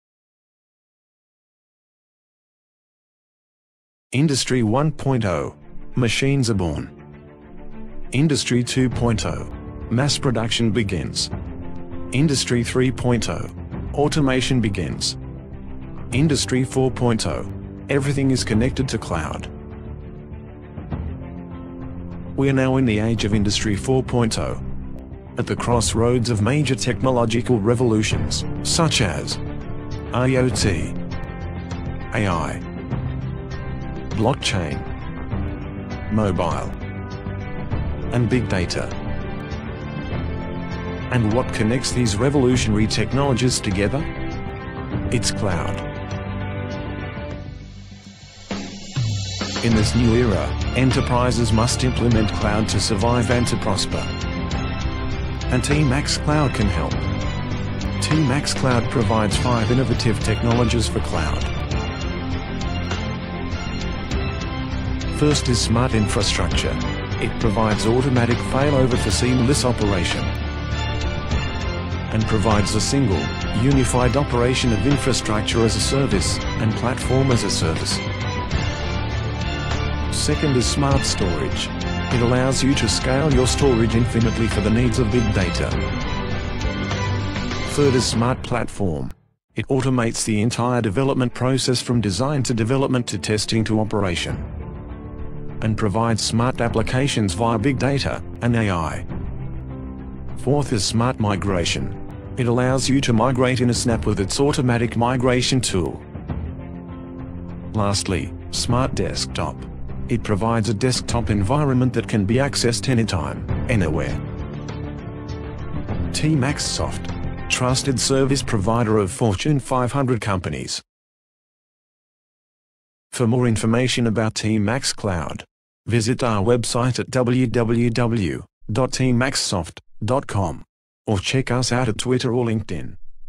TmaxCloud-The Right Foundation for Enterprise Transformation
TmaxCloud is a smart cloud services platform offering big data, AI, database storage, OS, and many other cloud-related technologies.
In this video, learn how enterprises can leverage TmaxCloud to increase their IT systems’ efficiency, scalability, flexibility, and reliability.